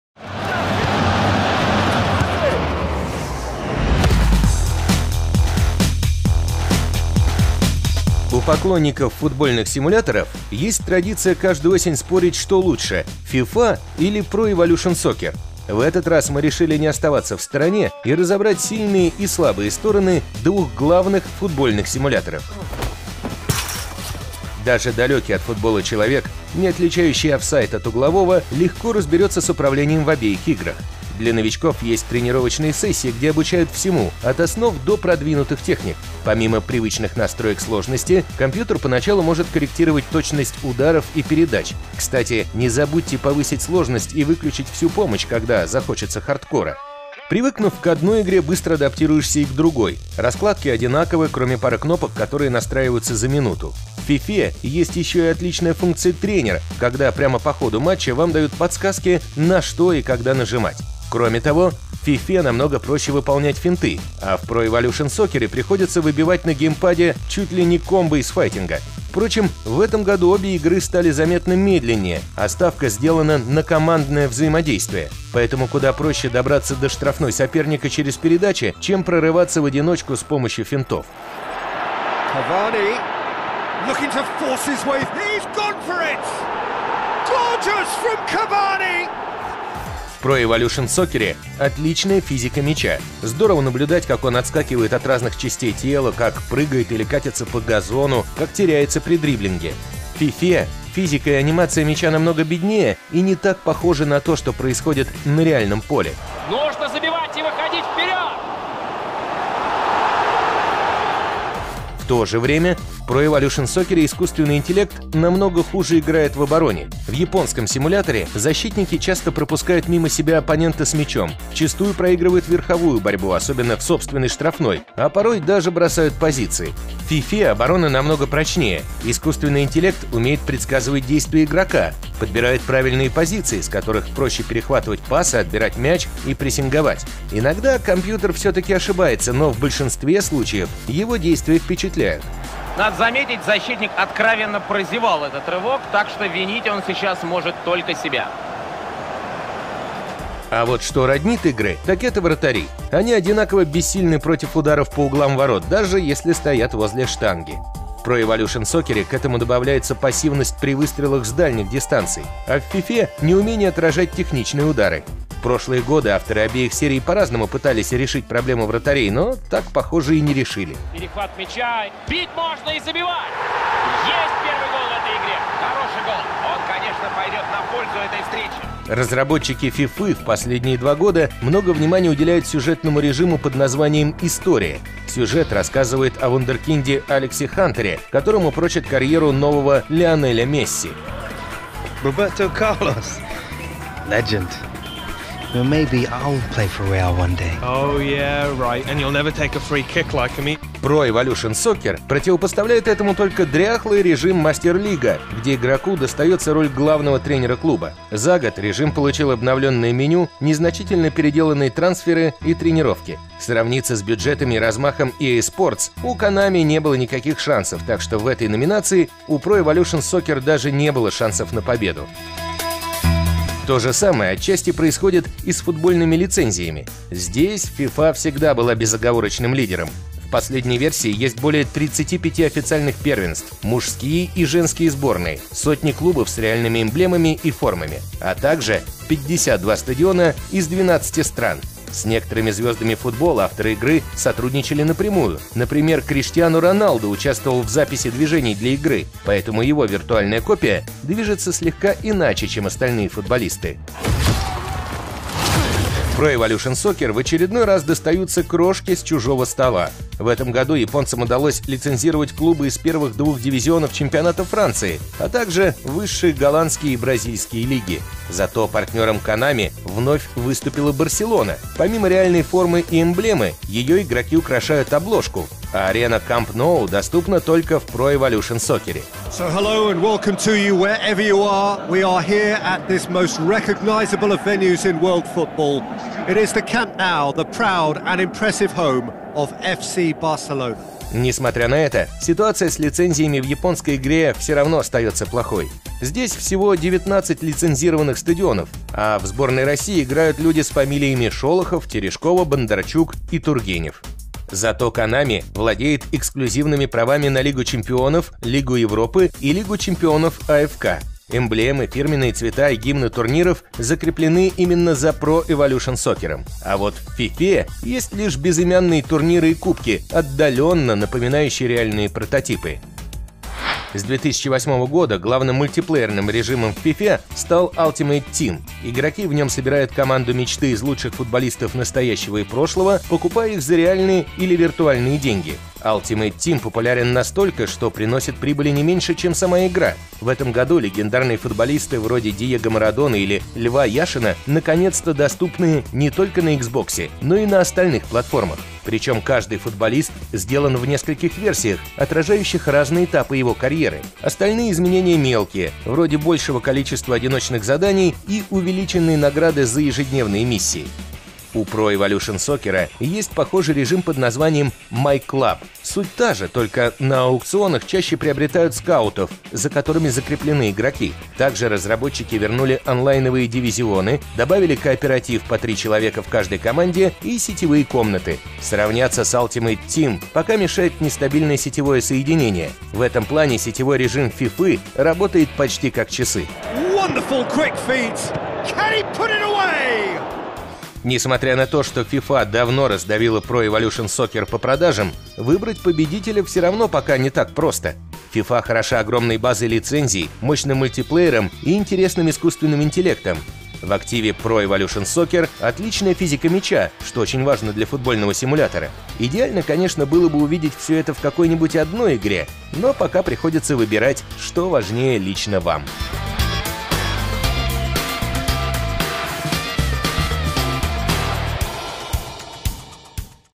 FIFA 18 против PES 2018
В какую игру податься поклоннику футбола? Сильные и слабые стороны двух главных футбольных симуляторов планеты